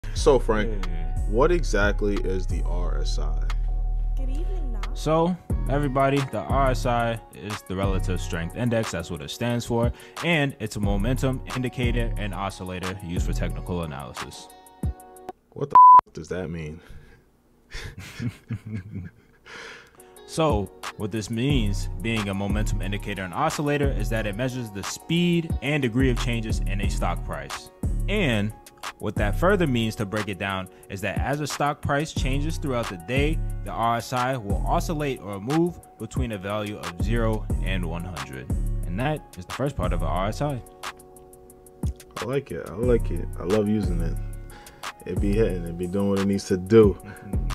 Stock Tips for Beginners (How to Use the RSI Pt. 1)
Video Description

Introducing the new Welcome to Wall Street: Stocks for Beginners series! In this short, Frank goes into the technical analysis side of investing and explains, for the beginners, what the RSI is. So sit back, take notes, and enjoy!

Cobbs Capital Socials